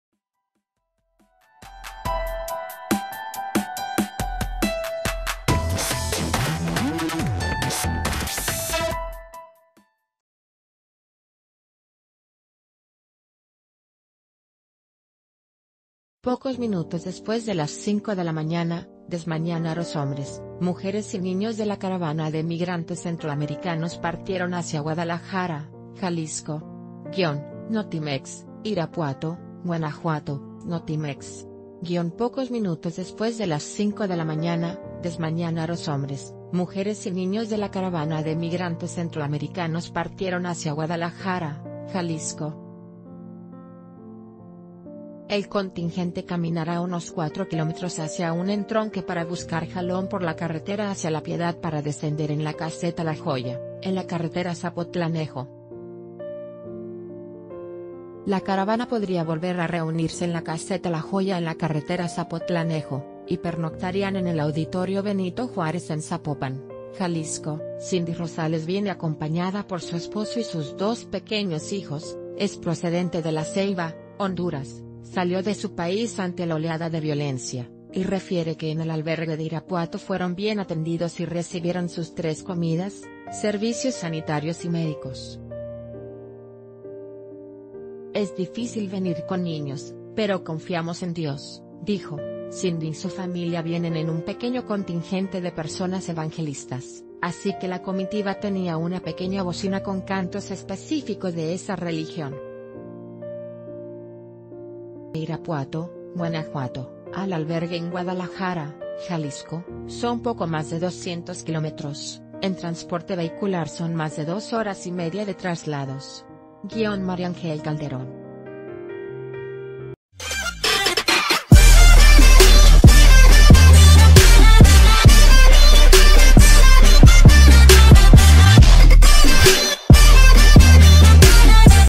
Caravana migrante se dirige hacia Guadalajara - El Diario de Yucatán
Pocos minutos después de las 5:00 de la mañana, desmañanados hombres, mujeres y niños de la caravana de migrantes centroamericanos partieron hacia Guadalajara, Jalisco.- (Notimex) IRAPUATO, Guanajuato (Notimex).- Pocos minutos después de las 5:00 de la mañana, desmañanados hombres, mujeres y niños d...